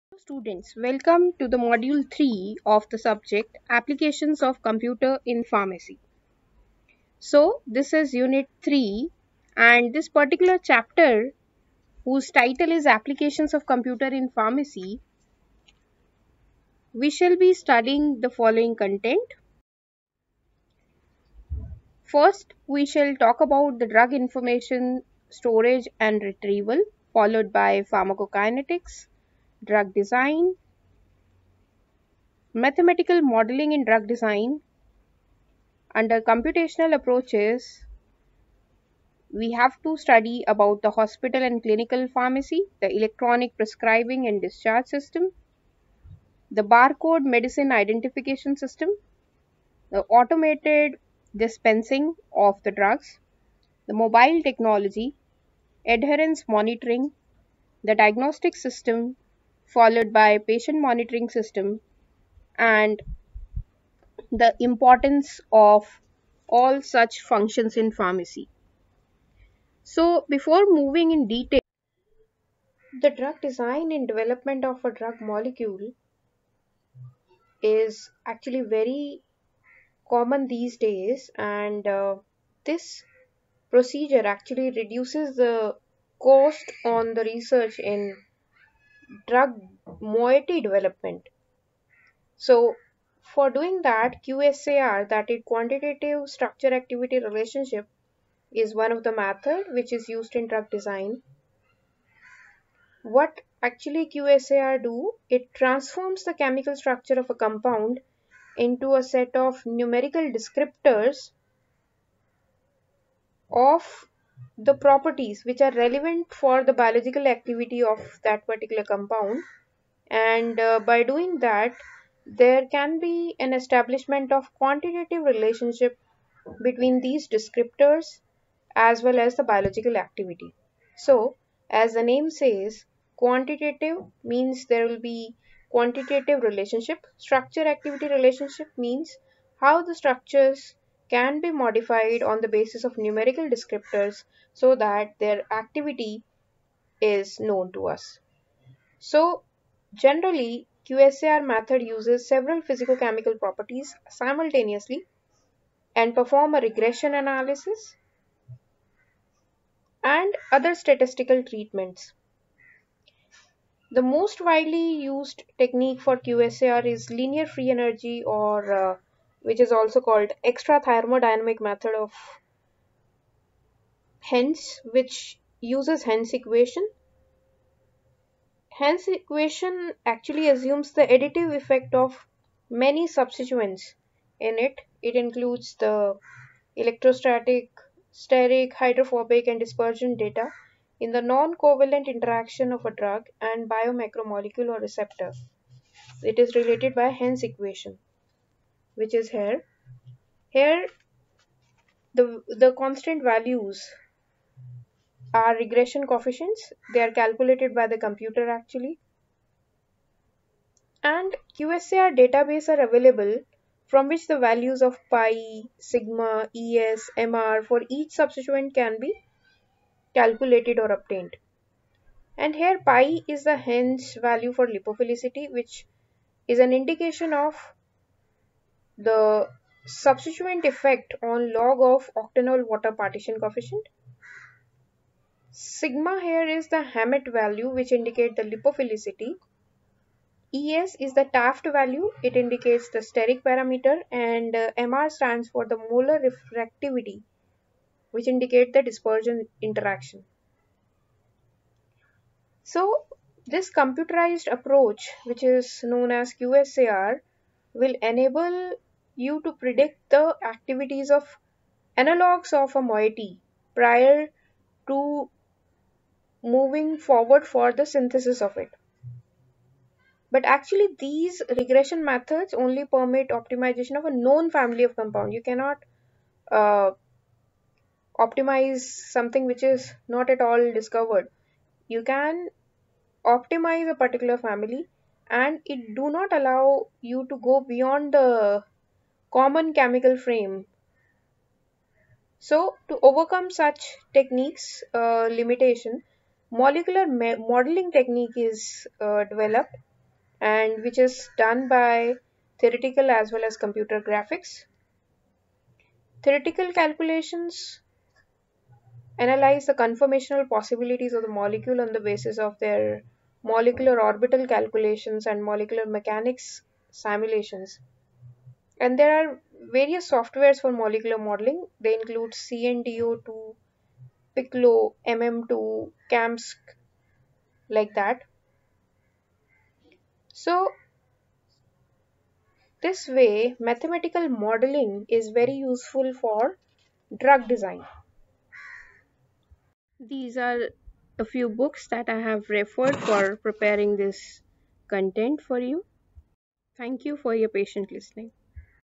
Module 3 Lecture 5 Mathematical Model for Drug Design
This video is about the application of computers for developing a Mathematical Model for Drug Design